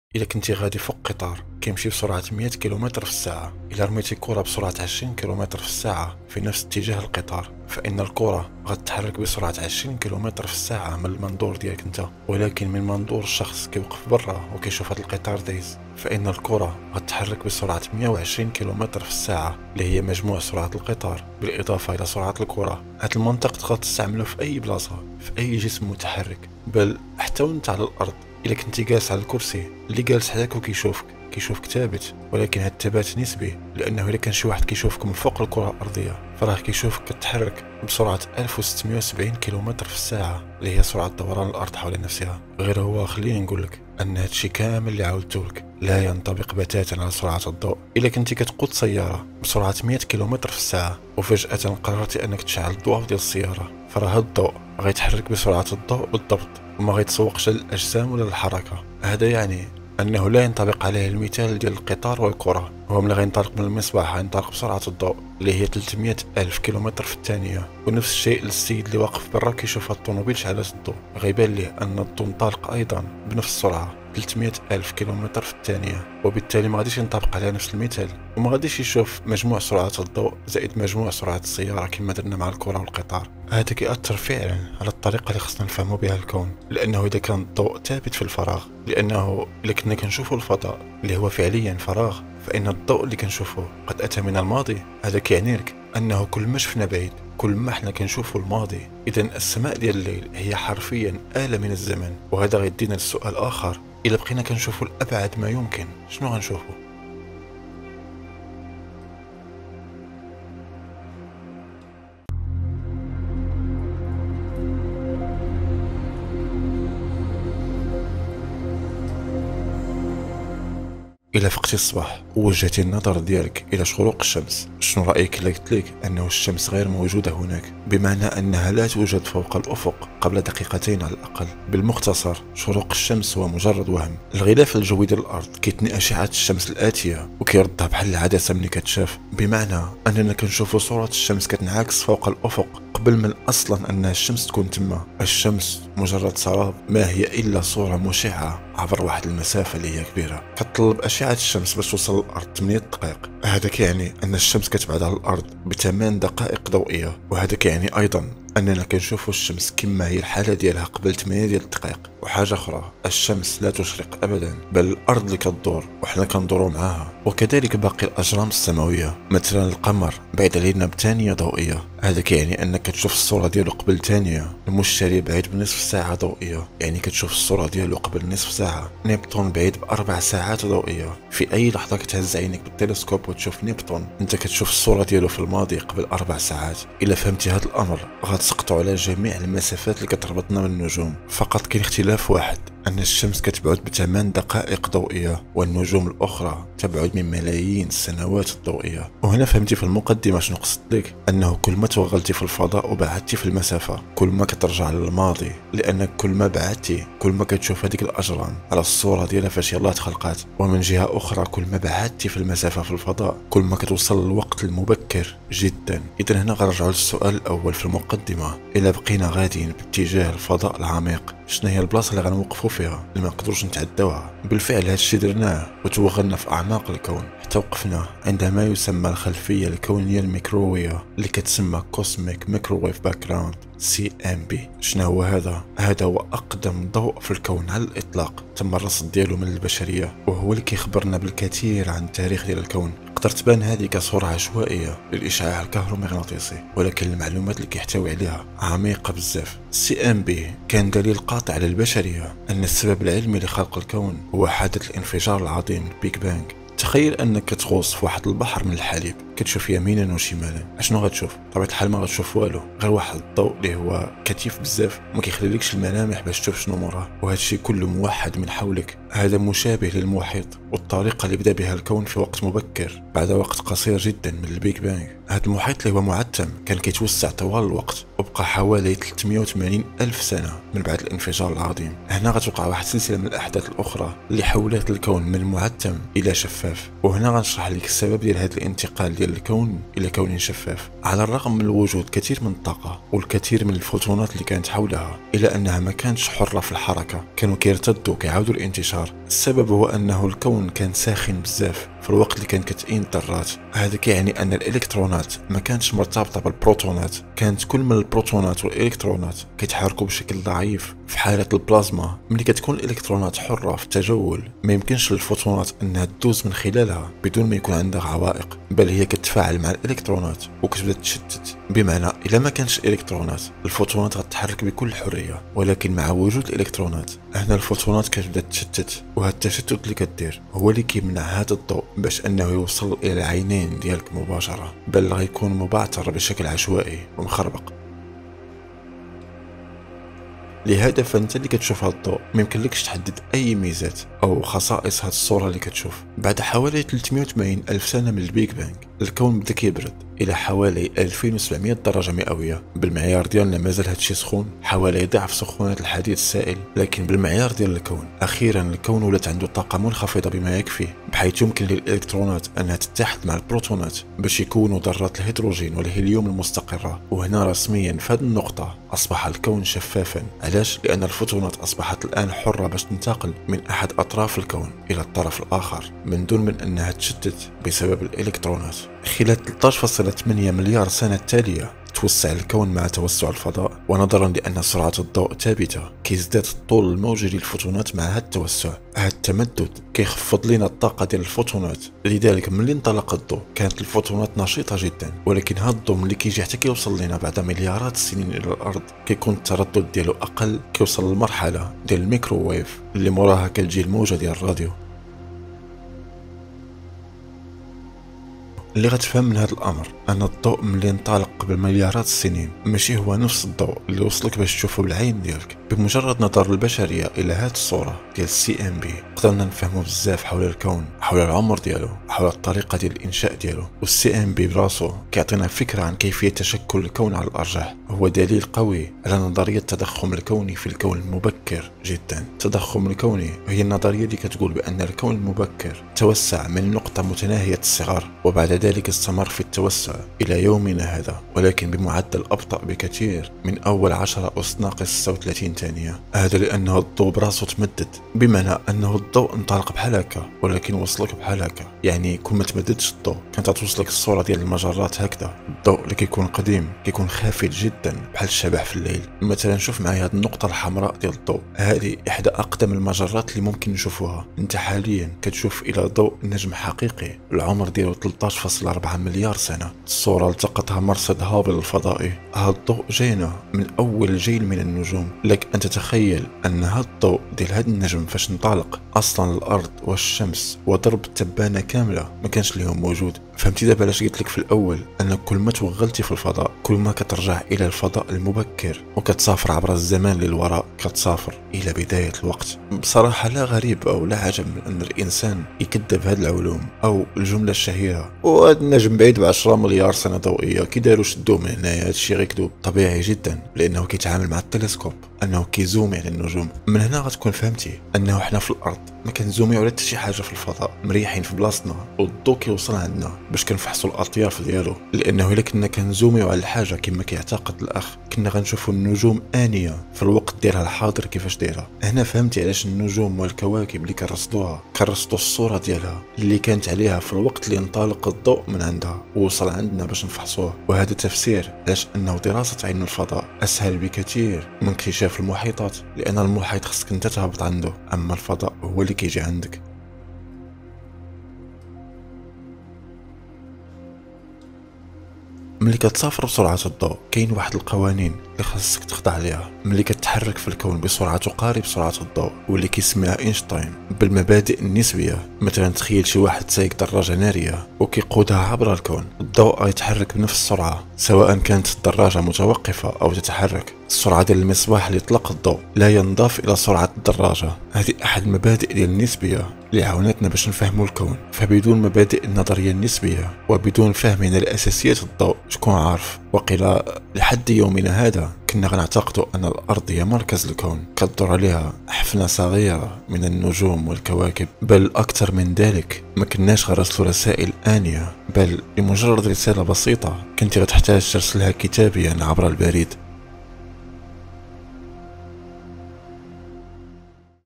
كيف عرفنا ماضي الكون دون أن نتواجد أنذاك؟ - النسبية و الضوء !
النظرية النسبية التي جاء بها البيرت انشتاين غيرت معالم العلم في العالم  فتحت للبشرية تصور اكبر و شامل للكن